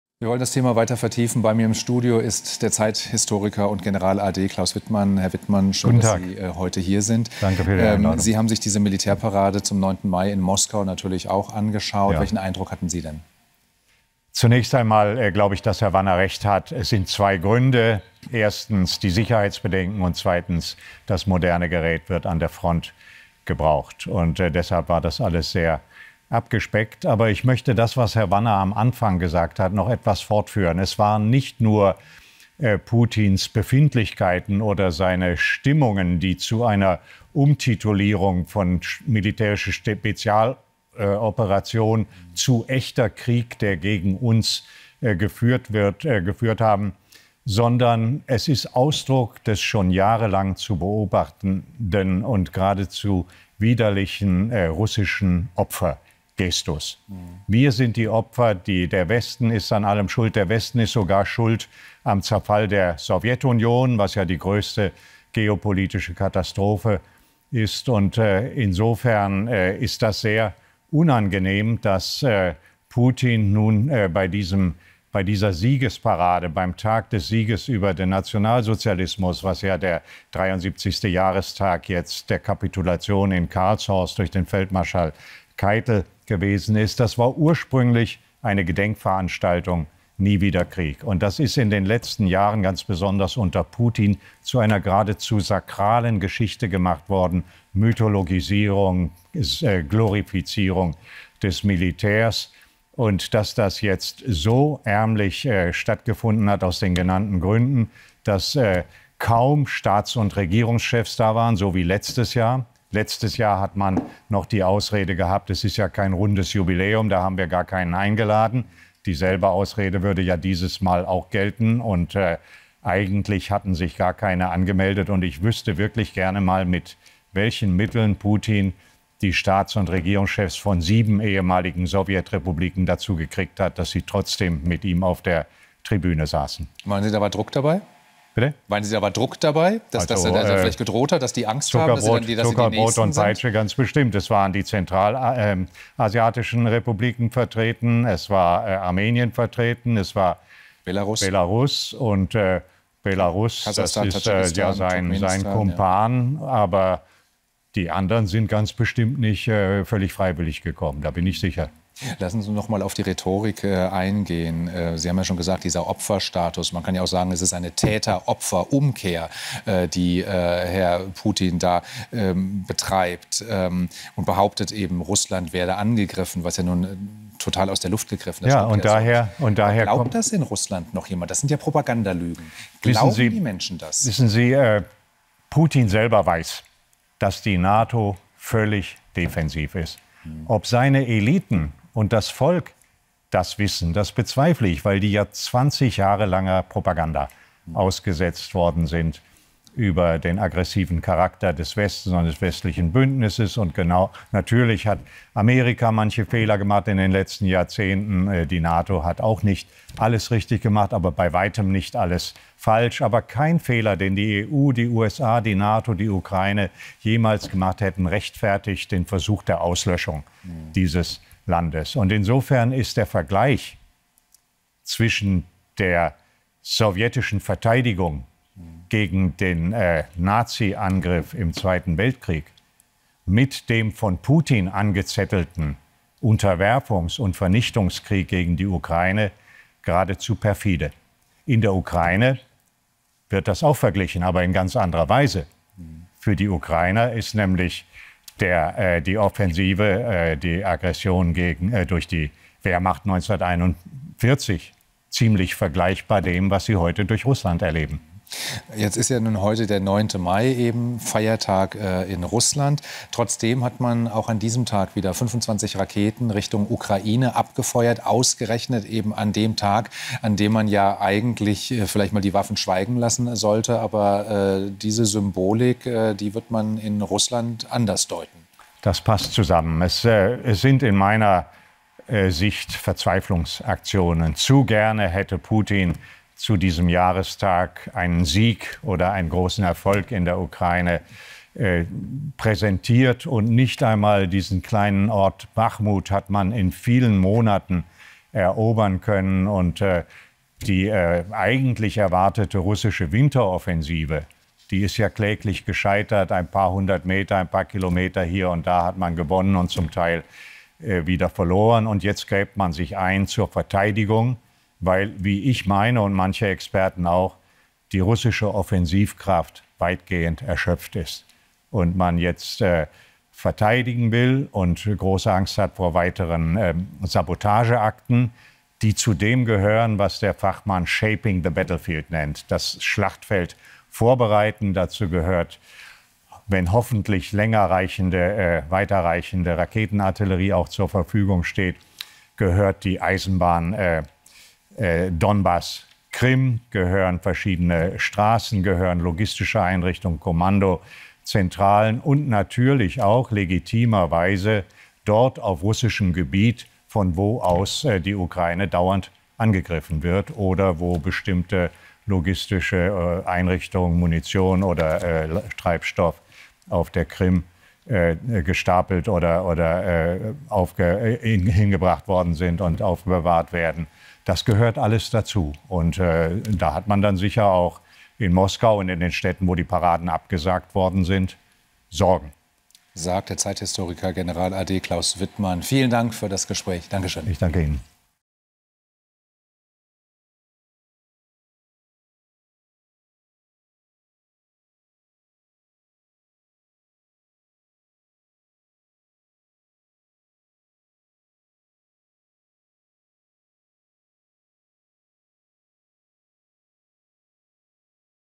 PUTINS TÄTER-OPFER-UMKEHR sei „Ausdruck des widerlichen, russischen Opfer-Gestus“ | Ukraine-Krieg
PUTINS TÄTER-OPFER-UMKEHR ist „Ausdruck des widerlichen russischen Opfer-Gestus“ | Ukraine-Krieg
Russland feiert am 09. Mai den Jahrestag des sowjetischen Sieges über Nazi-Deutschland. 8000 Soldaten liefen auf, Panzer rollten über den Roten Platz in Moskau. General a.D. und Militär-Historiker Klaus Wittmann spricht im WELT-Interview über den Aufmarsch.